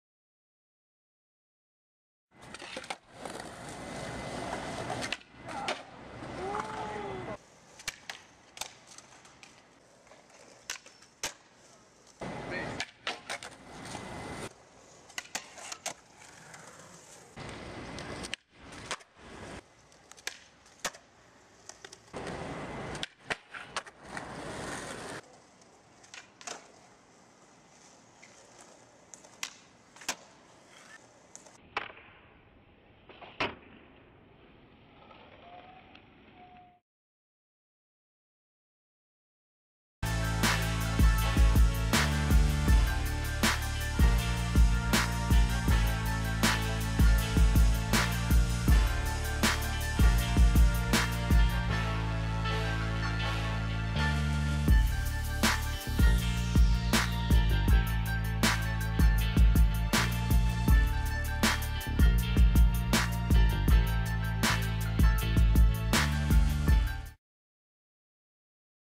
Skateboarding quick clips at Monrovia skate park Eljay Mclucas
Eljay Mclucas skating around the Monrovia skate park, filmed by Doug Des Autels